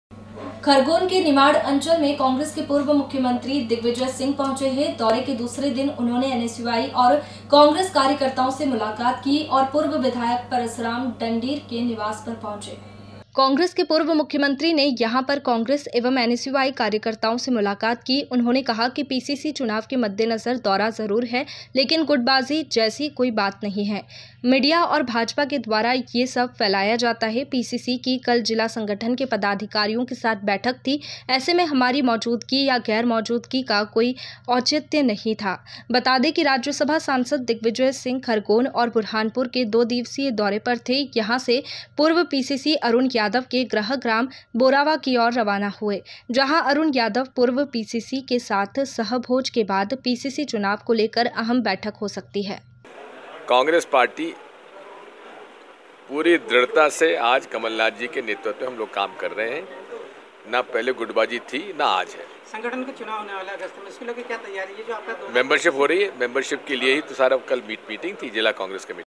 खरगोन | कांग्रेस के पूर्व मुख्यमंत्री व राज्य सभा सांसद खरगोन पहुंचे | पीसीसी की बैठक में होगे शामिल
कांग्रेस के पूर्व मुख्यमंत्री ने  यहां पर कांग्रेस एवं एनएसयूआई कार्यकर्ताओं से मुलाकात की ।  उन्होंने कहा कि पीसीसी चुनाव के मद्देनजर दौरा जरूर है लेकिन गुटबाजी जैसी कोई बात नहीं है,  मीडिया और भाजपा के द्वारा यह सब फैलाया जाता है । पीसीसी की कल जिला संगठन के पदाधिकारियों के साथ बैठक थी ,  ऐसे में हमारी मौजूदगी या गैरमौजूदगी का कोई औचित्य नहीं था।  बता दें कि राज्यसभा सांसद दिग्विजय सिंह खरगोन और बुरहानपुर के दो दिवसीय दौरे पर थे यहां से पूर्व पीसीसी अरुण यादव के गृह ग्राम बोरावा की ओर रवाना हुए ।  जहां अरुण यादव पूर्व पीसीसी के साथ सहभोज के बाद पीसीसी चुनाव को लेकर अहम बैठक हो सकती हैं।

बाईट। दिग्विजय सिह
पूर्व मुख्यमंत्री